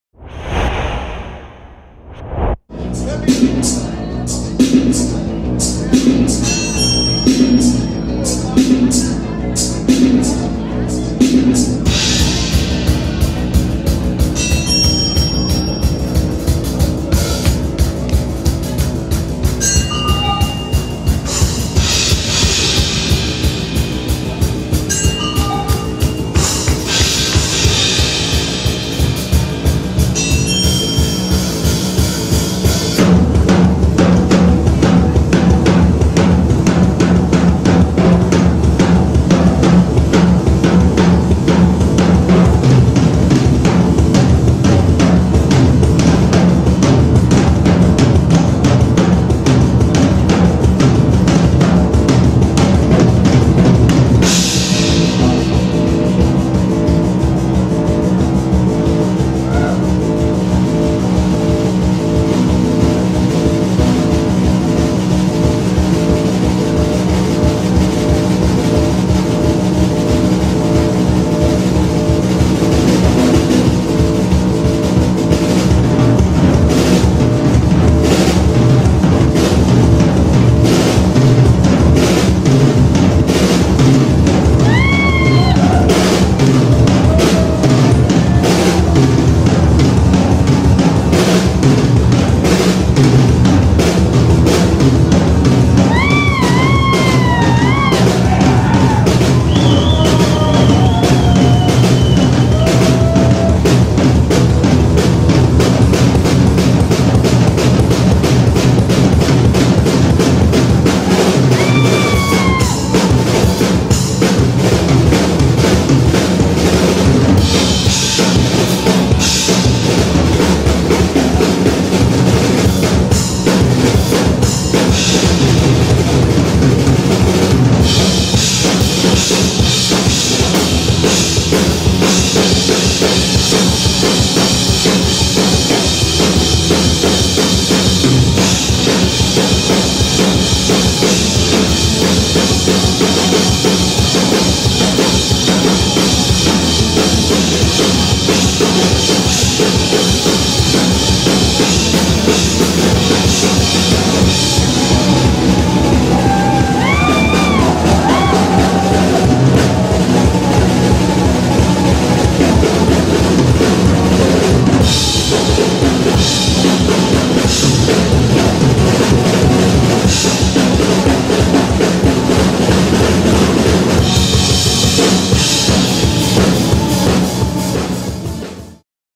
Drum Solo Niko Cabrera - Doctor Krapula en Bielefeld - Alemania
Drum solo de la gira Corazón Bombea Europa, en compañía de Sergio Acosta en los teclados.